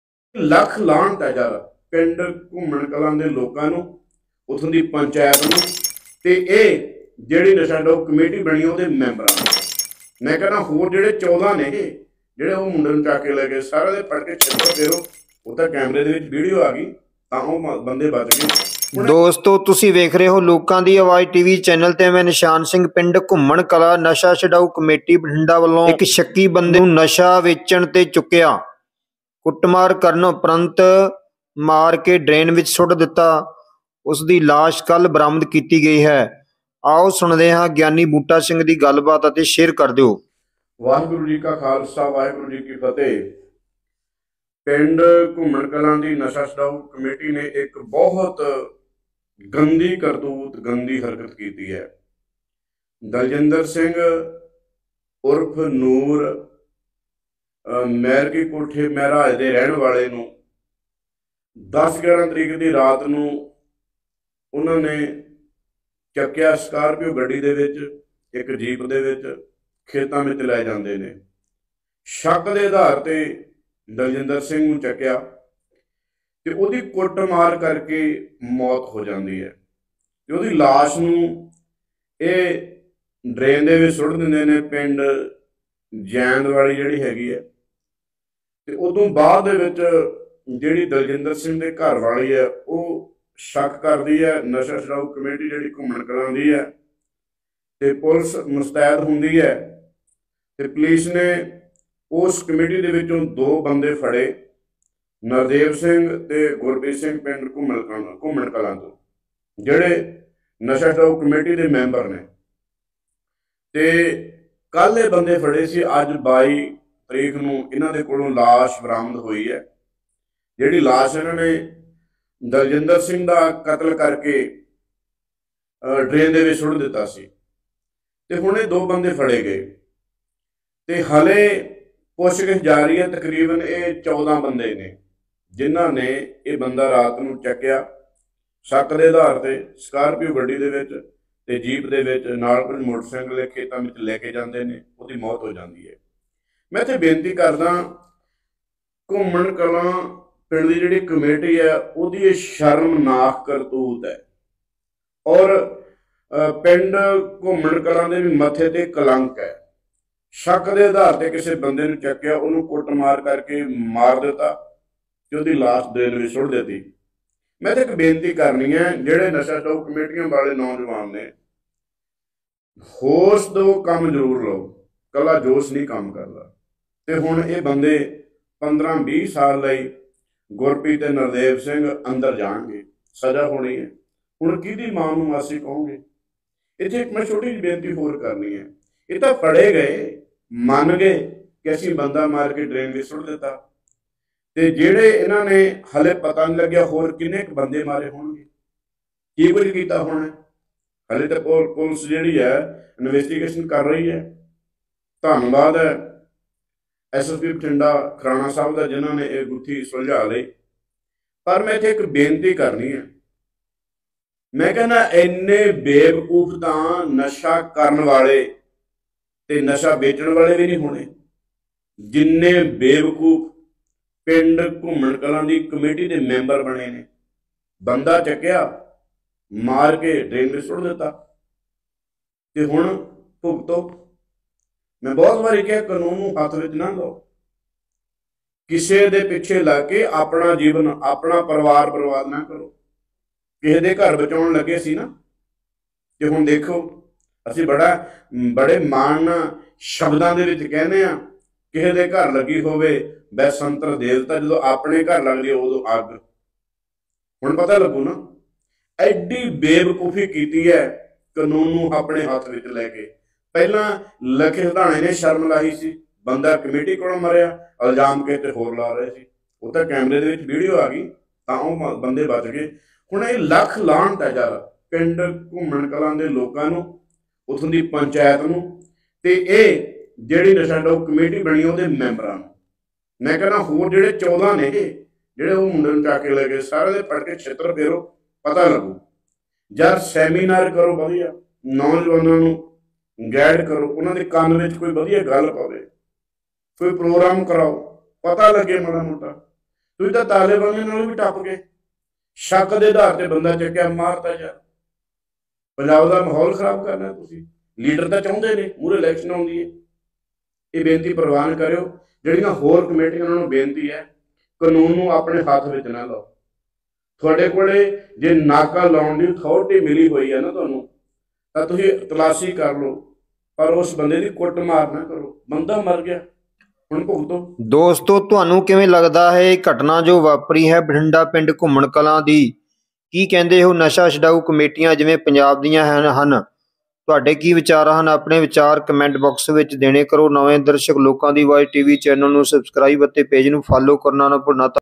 ਬੰਦਾ ਮਾ/ਰ ਕੇ ਡਰੇਨ ਚ ਸੁੱਟਿਆ bathinda news today punjab news today
ਨ/ਸ਼ਾ ਕਮੇਟੀ ਨੇ ਬੰਦਾ ਮਾ/ਰ ਕੇ ਡਰੇਨ ਚ ਸੁੱਟਿਆ bathinda news today punjab news today Lokan di awaz tv